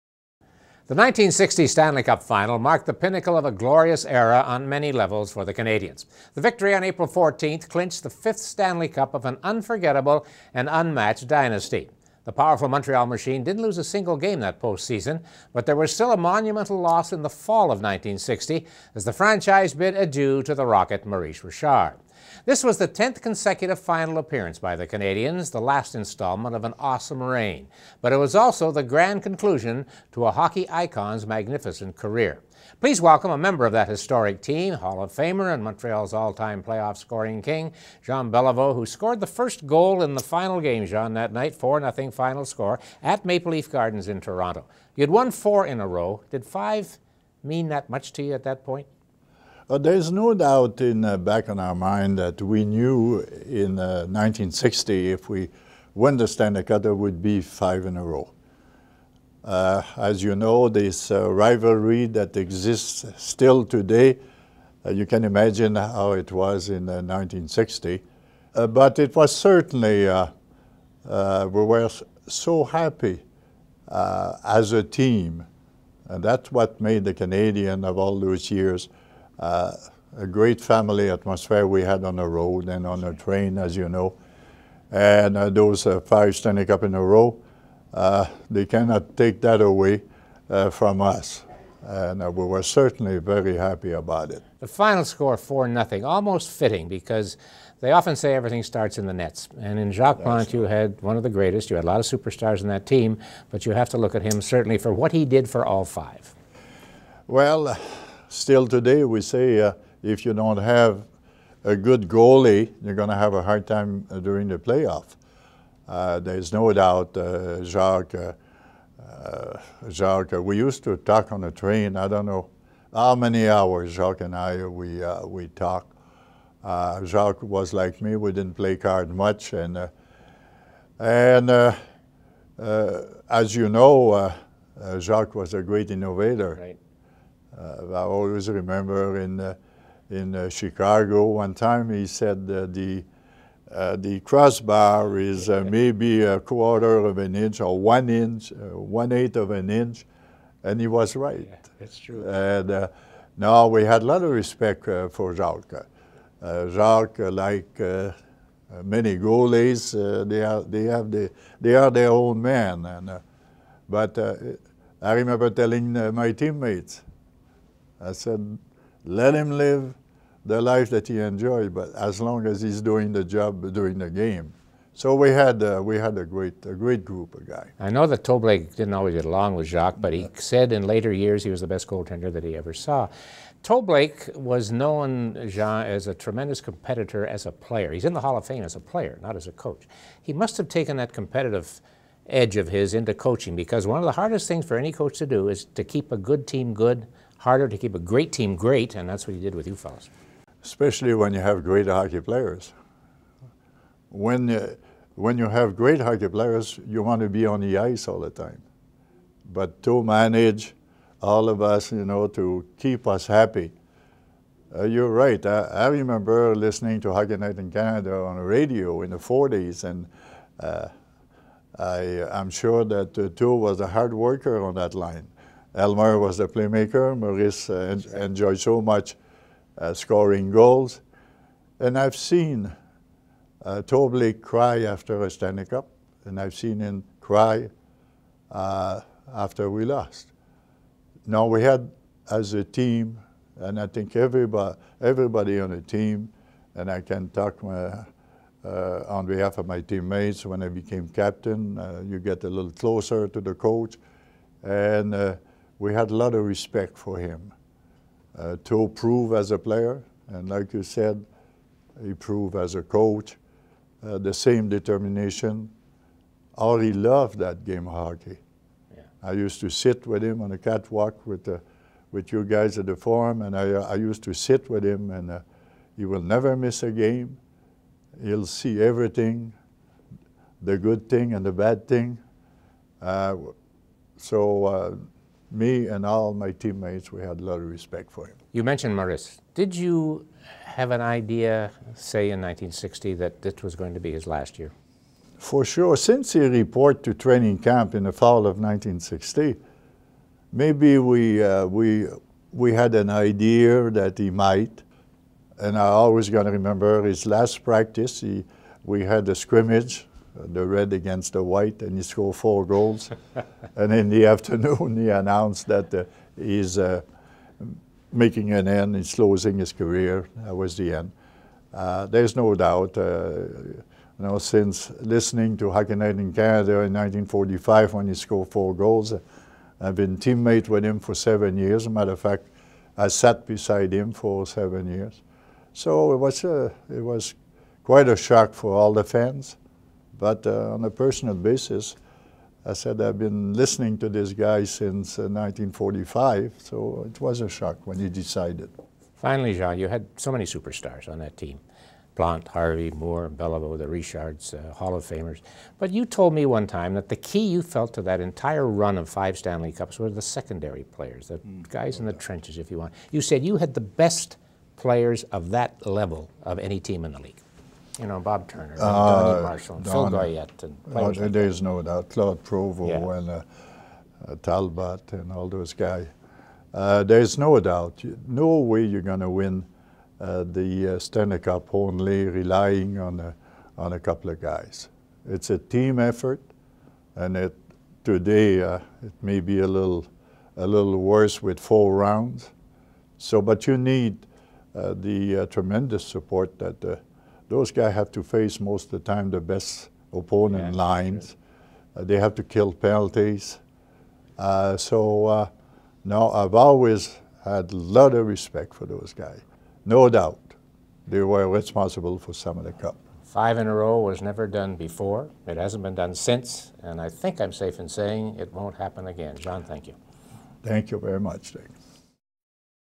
Jean Beliveau Interview
Jean Beliveau being interviewed by Dick Irvin